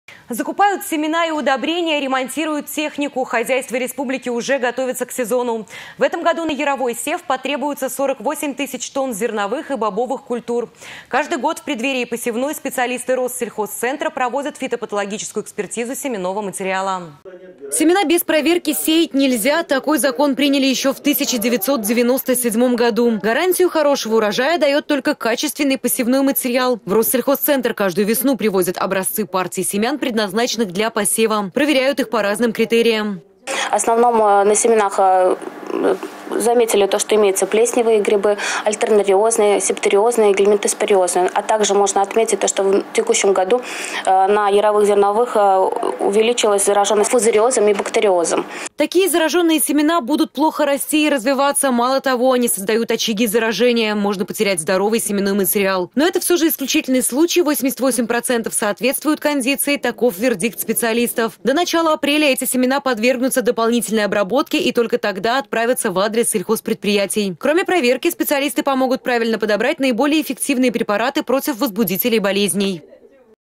Хозяйства республики готовятся к посевной
Закупают семена и удобрения, ремонтируют технику -  хозяйства республики уже готовятся к сезону. В этом году на яровой сев потребуется 48 тысяч тонн зерновых и бобовых культур. Каждый год в преддверии посевной специалисты «Россельхозцентра» проводят фитопатологическую экспертизу семенного материала.